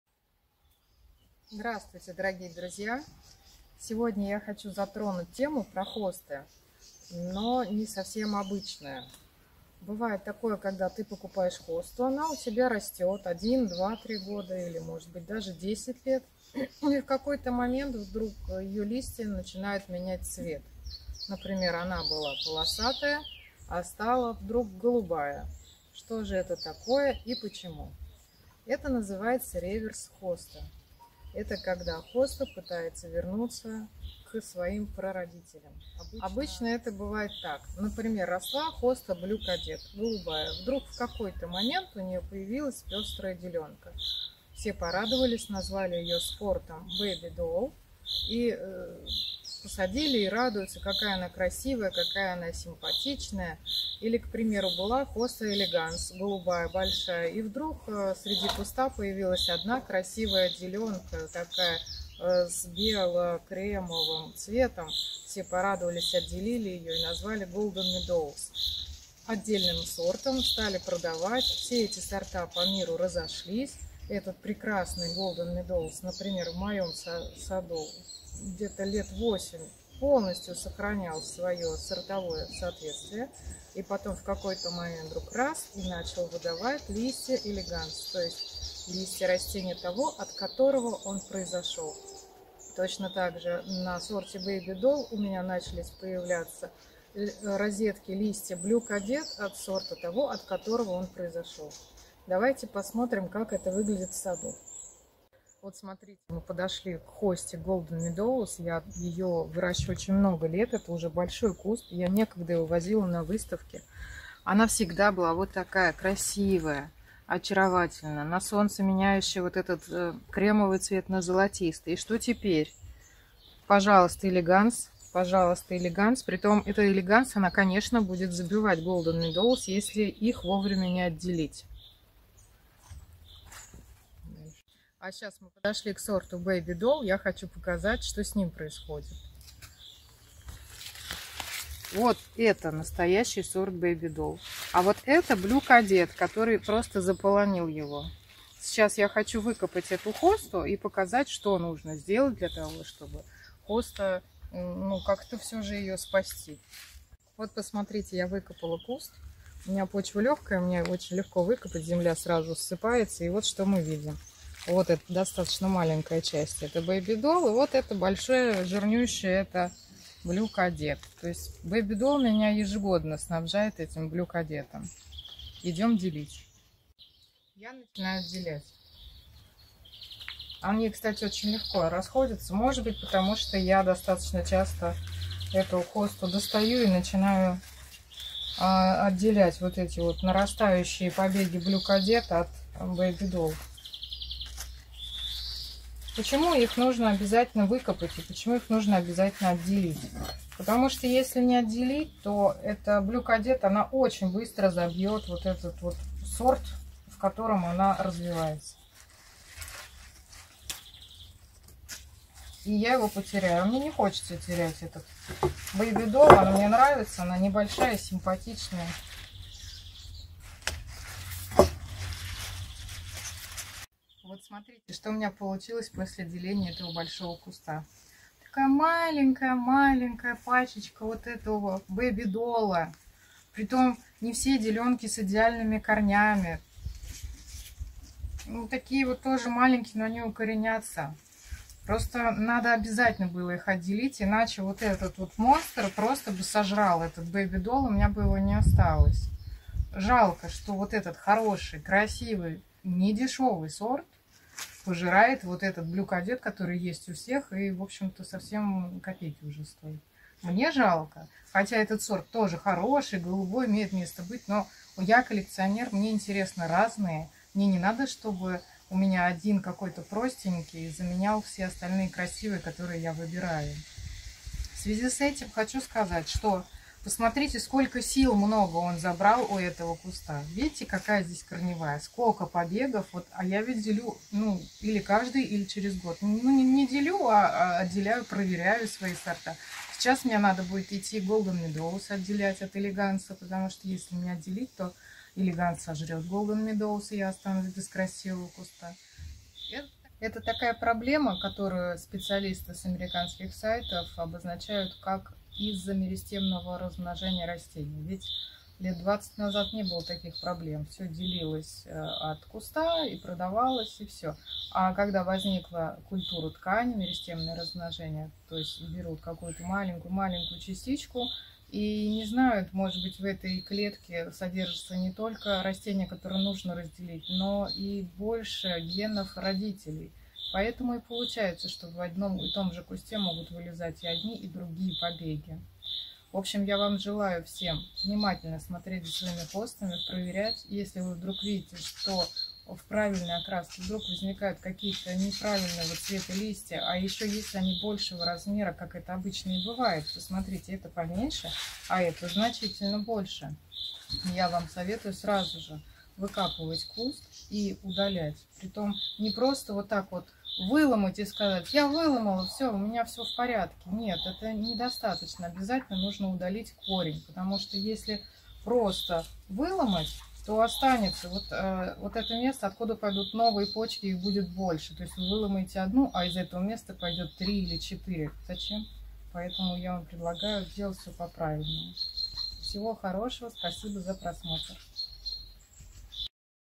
Реверс хосты. Что делать, если ваши хосты поменяли окрас листа
Продолжая тему хост в этом видео я расскажу о таком явлении как реверс хосты. Оно будет в первую очередь интересно большим любителям и ценителям этих многолетников, которые сталкиваются с редким, но иногда появляющимся явлении в своем саду, как реверс хост.
Это явление можно заметить на хостах в тот момент, когда среди обычных листьев начинают вылезать листья совершенно другого окраса, листья совершенно другого сорта.
Происходит это по причине того, что куст хосты ни с того, ни с сего начинает воспроизводить листья своего прородителя, сорта из которого была получена эта хоста.
Как правило старый сорт намного проще и не так ярок, поэтому я расскажу и покажу что делать в этой ситуации и как избавиться от непрошенного гостя.